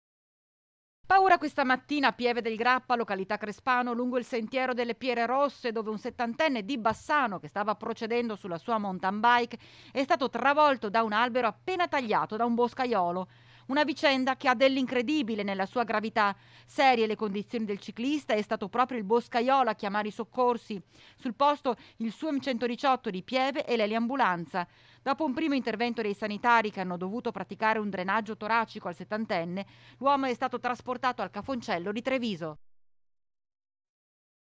CICLISTA TRAVOLTO DA ALBERO TAGLIATO DA UN BOSCAIOLO | 09/01/2021
TG Bassano del Grappa 09/01/2021 - PIEVE DEL GRAPPA - Boscaiolo taglia un albero che travolge e ferisce un ciclista di 70 anni. L'uomo trasportato d'urgenza con l'elisoccorso a Treviso || Paura questa mattina a Pieve del Grappa, località Crespano, lungo il sentiero delle Piere Rosse, dove un 70enne di Bassano, che stava procedendo sulla sua mountain bike è stato travolto da un albero appena tagliato un boscaiolo. Una vicenda che ha dell'incredibile nella sua gravità. Serie le condizioni del ciclista. E' stato proprio il boscaiolo a chiamare i soccorsi. Sul posto il Suem 118 di Pieve e l'eliambulanza. Dopo un primo intervento dei sanitari, che hanno dovuto praticare un drenaggio toracico al 70enne, l'uomo è stato trasportato al Cà Foncello di Treviso. (Servizio di Vanessa Banzato) - Segui Rete Veneta anche sul digitale terrestre! Visita il sito